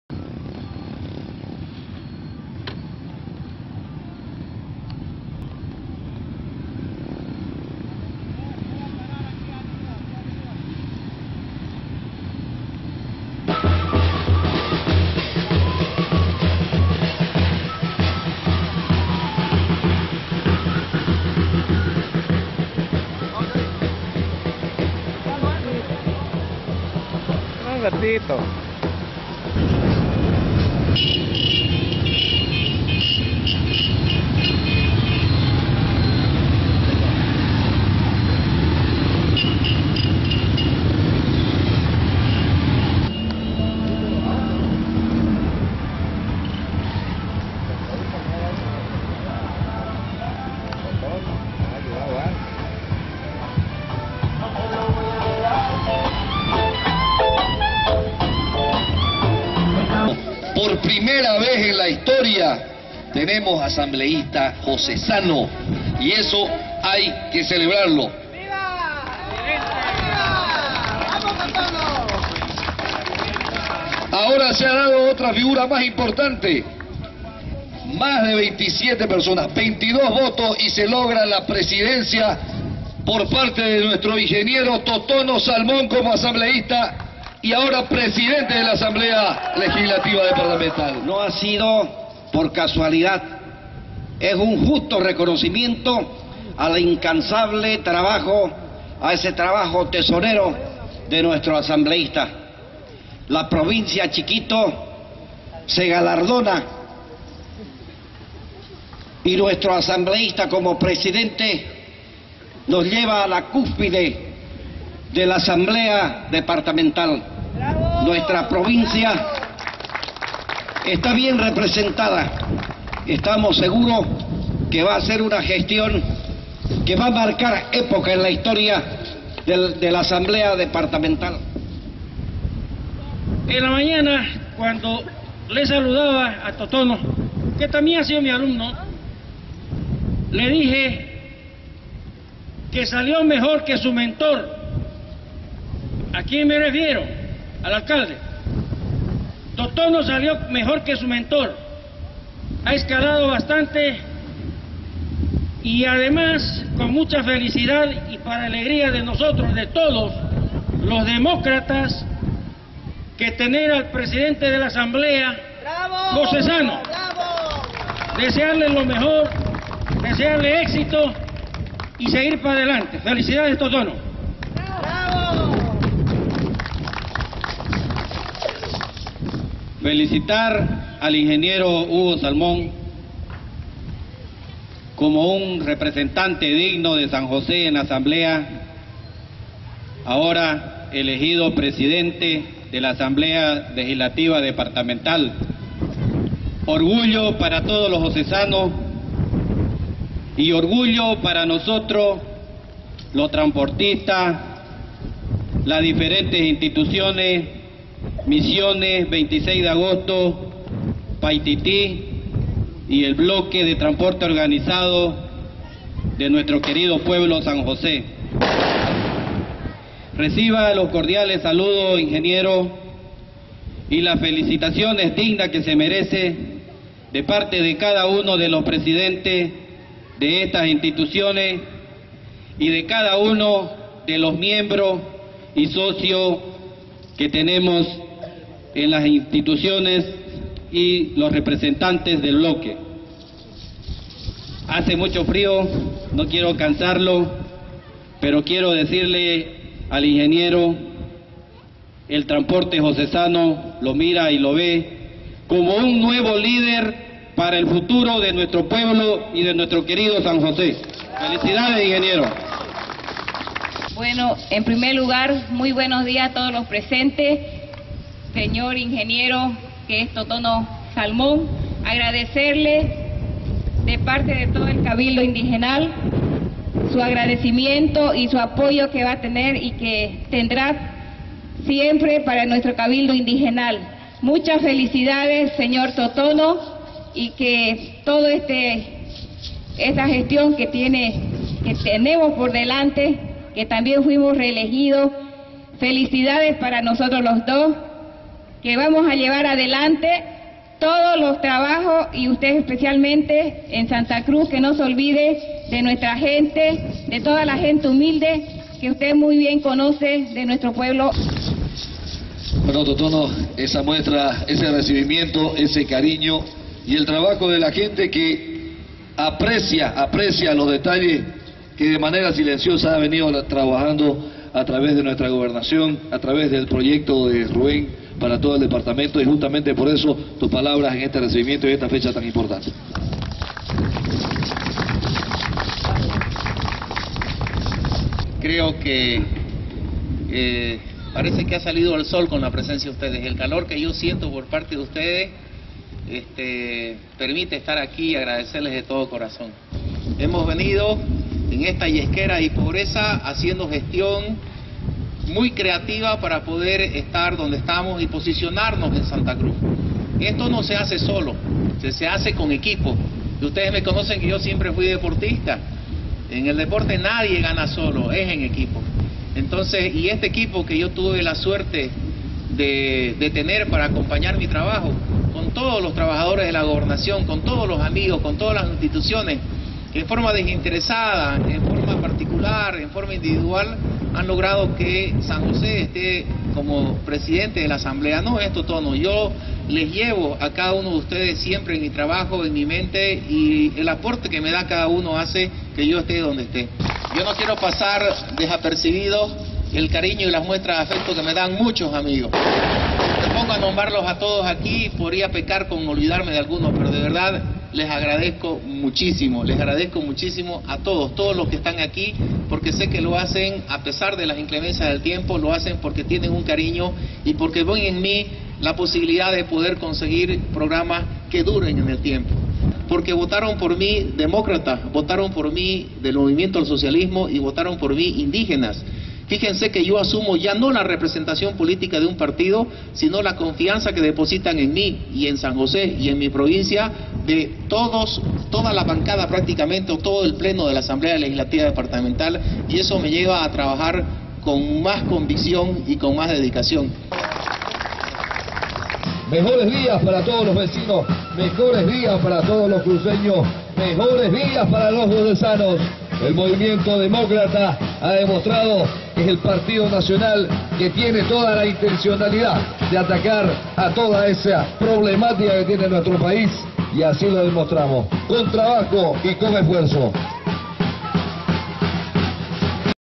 Caravana de recibimiento al presidente de la Asamblea Legislativa Departamental de Santa Cruz
Por primera vez en la historia un josesano es Presidente de la Asamblea Legislativa Departamental. Su nombre Hugo Totono Salmon quien fue recibido por.los vecinos de San Jose en una gran caravana que culmino en un acto a los pies de la Torre de Campanario Jesuitico en la Plaza principal de San Jose de Chiquitos.